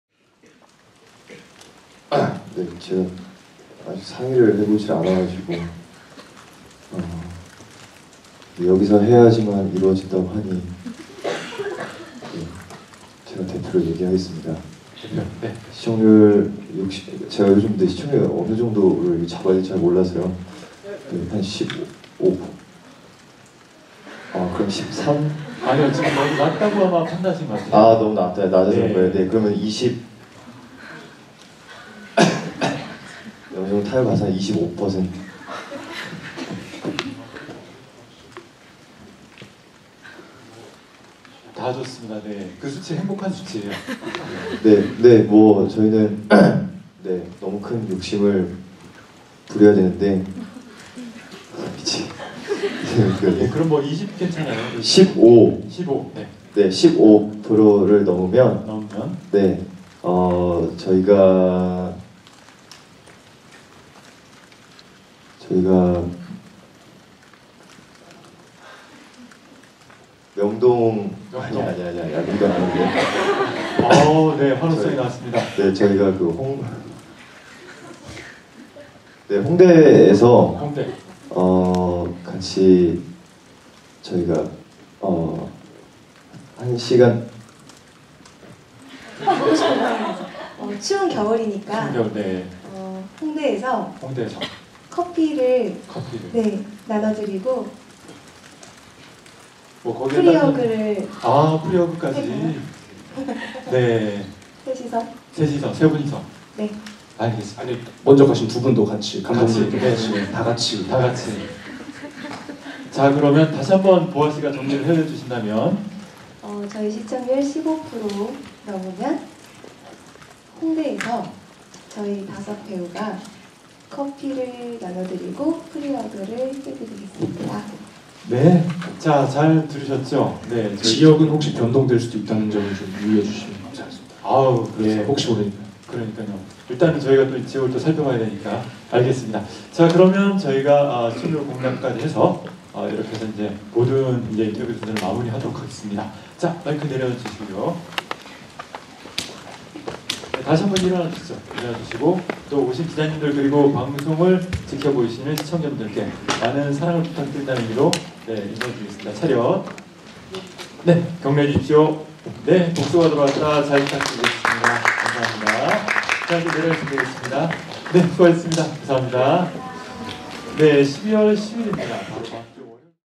유승호(Yoo Seung Ho) 복수가 돌아왔다 시청률 공약 "따뜻한 커피와 프리허그!"
12월 7일 오후 서울 양천구 목동 SBS홀에서 SBS 월화 드라마 ‘복수가 돌아왔다(연출 함준호, 극본 김윤영)’ 제작발표회가 열렸다. 드라마 ‘복수가 돌아왔다’는 학교 폭력 가해자로 몰려 부당하게 퇴학을 당한 강복수가 어른이 돼 학교로 다시 돌아가 복수를 계획하지만, 복수는 고사하고 또다시 사건에 휘말리고 사랑도 다시 하는 엉뚱하면서 따뜻한 감성 로맨스물로 12월 10일 밤 10시 첫 방송된다.